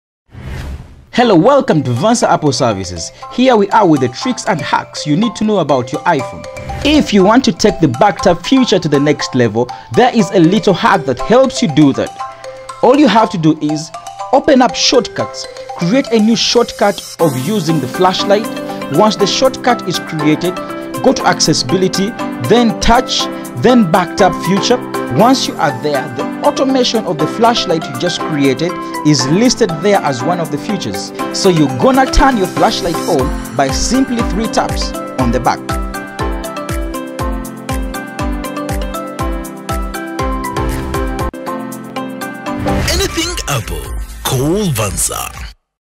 Back Tap Feature Trick on iPhone. FLASHLIGHT OPTION
here is a video to teach you how to enable flashlight with a back tap for iphone users.